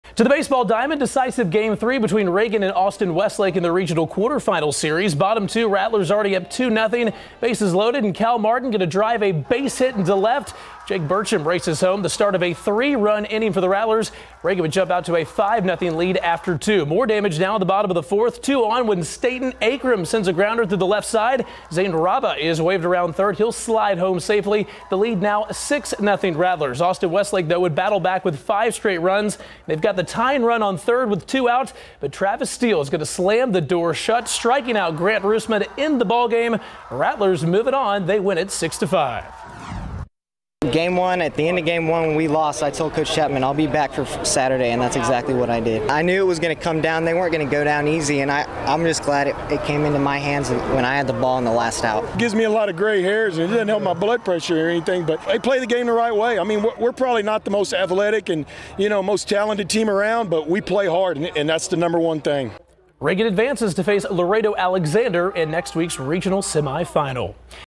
Reagan tops Austin Westlake to advance
The Reagan Rattlers topped Austin Westlake 6-5 to win the decisive Game 3 of the teams' regional quarterfinal series at North East Sports Park